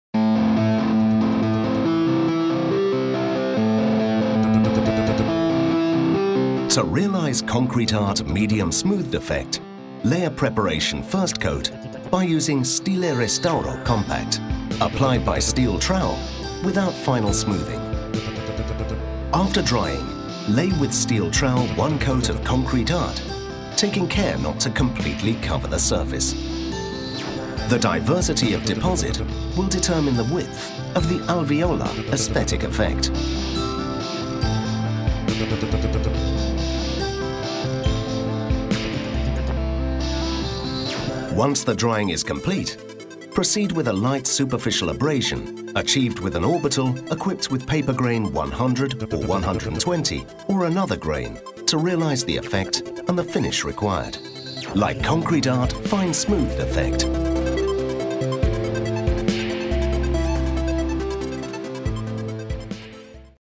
Concret_Art Sa Srednje Zagladenim i Fino (Hrvatski- Srpski)
CONCRET_ART je unutarnji dekorativni završni premaz sa izgledom cementa sa kojim je moguće reproducirati opipljiv, solidan izgled prirodno ostarjelog i proživljenog cementa te prenijeti tražene detalje. Ova video snimka sadrži naše savjete za realizaciju "SREDNJE ZAGLAĐENOG" i "FINO ZAGLAĐENOG" izgleda.